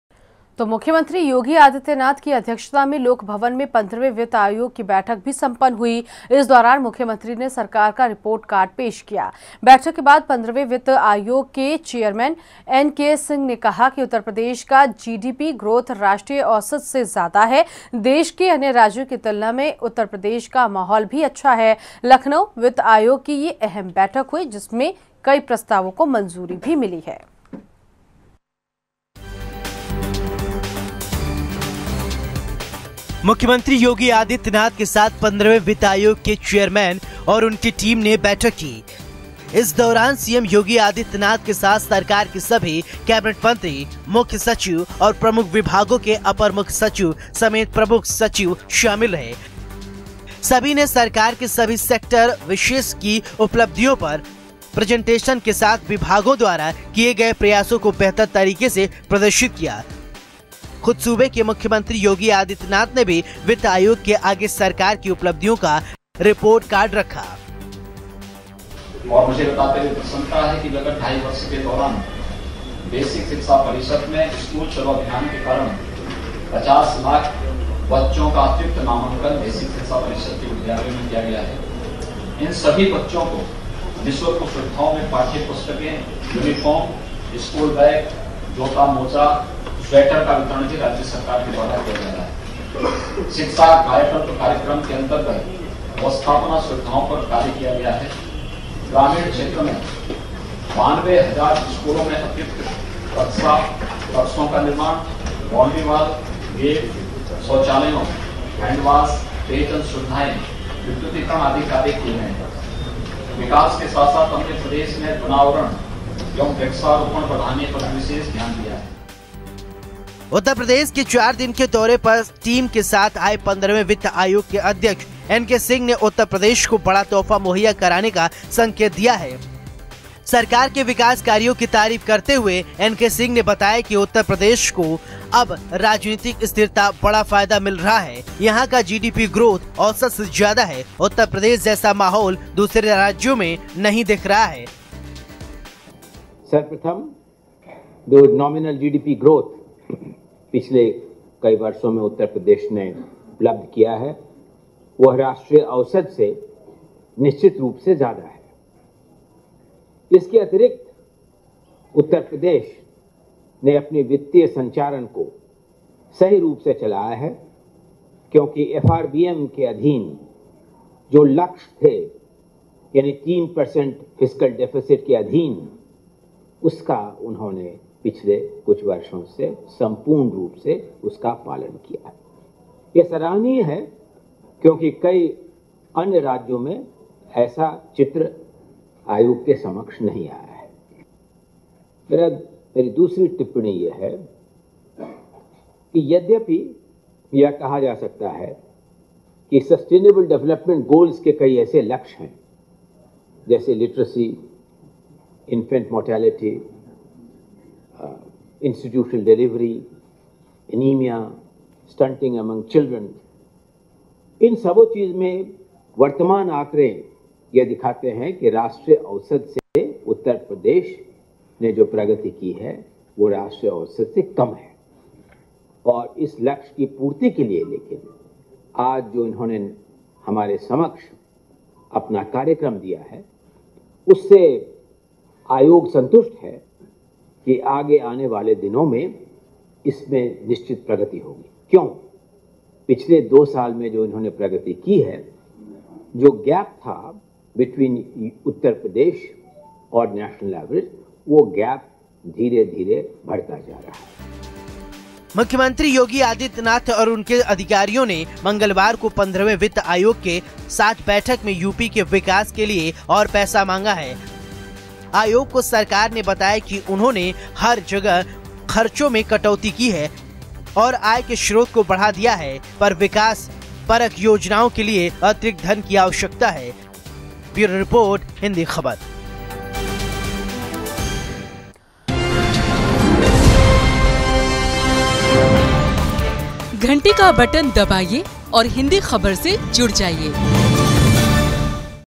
सीएम योगी ने सरकार की गिनाई उपलब्धियां, वित्त आयोग के अध्यक्ष ने की सरकार की तारीफ
मुख्यमंत्री योगी आदित्यनाथ की अध्यक्षता में लोकभवन में 15वें वित्त आयोग की बैठक संपन्न हो गई. इस दौरान मुख्यमंत्री ने सरकार का रिपोर्ट कार्ड पेश किया... बैठक के बाद 15वें वित्त आयोग के चेयरमैन एनके सिंह ने कहा कि उत्तर प्रदेश का जीडीपी ग्रोथ राष्ट्रीय औसत से ज्यादा है... देश के अन्य राज्यों की तुलना में उत्तर प्रदेश का माहौल अच्छा है.